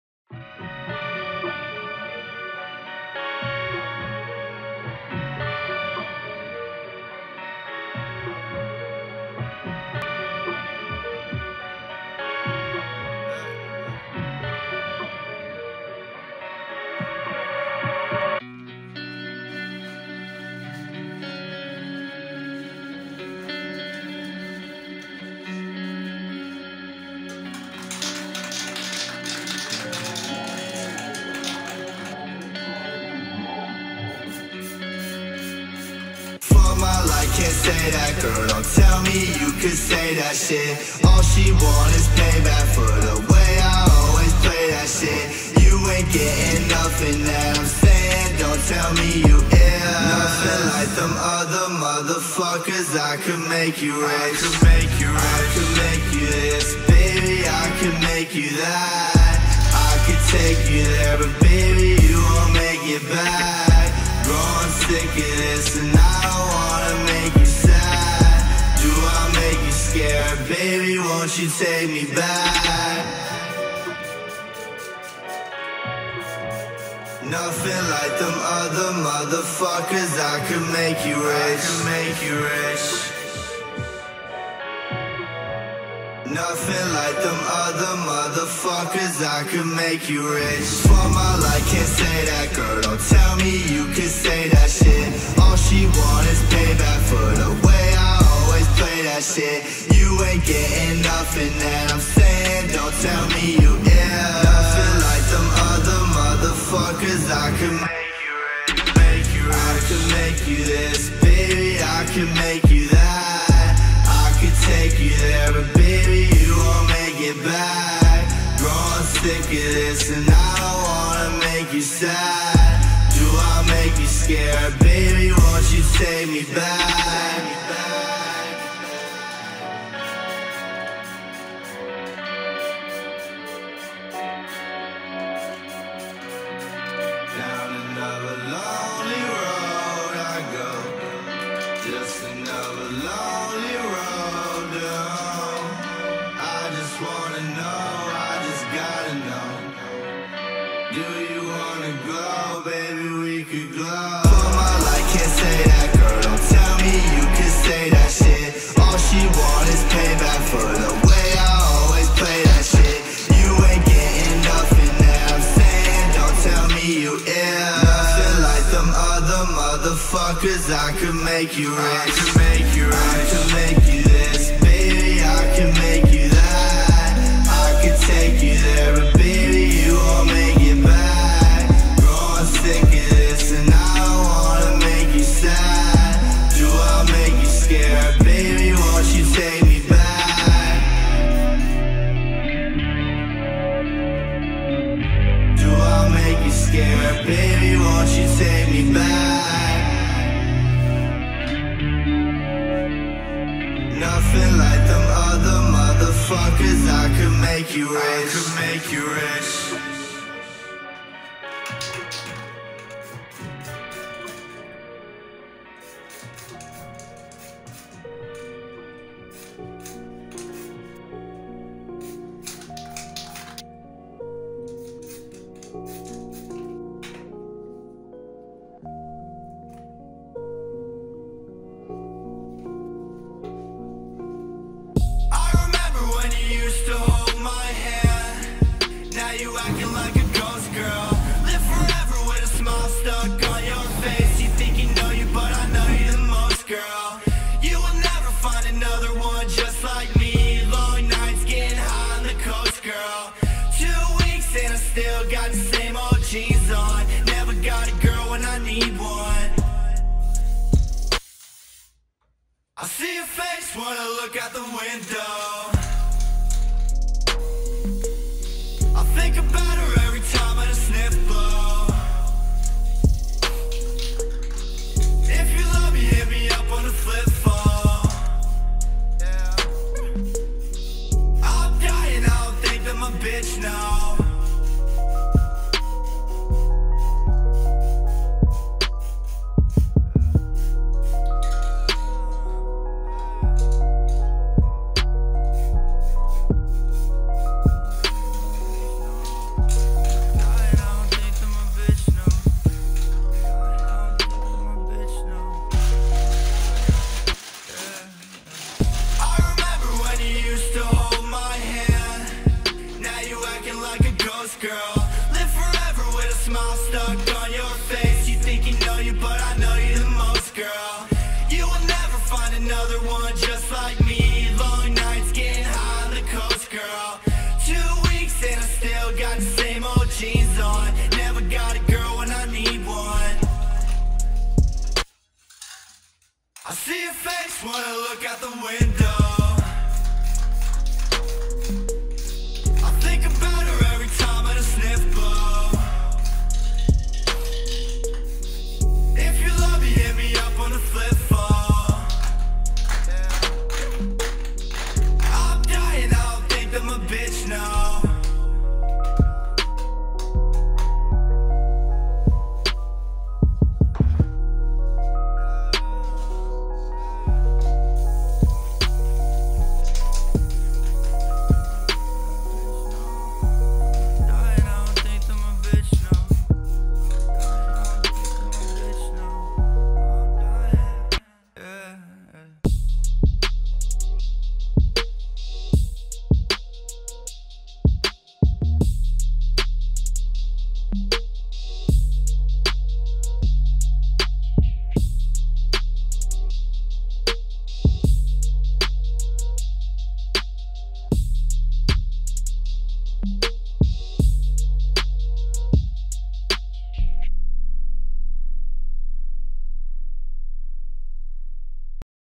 PAINT SPRAY ART#1 LIL PEEP VEN4ESTER
Hello! This is the first video about paint spray art. I decided to portray Gustav Ahr because I am a longtime fan of his work.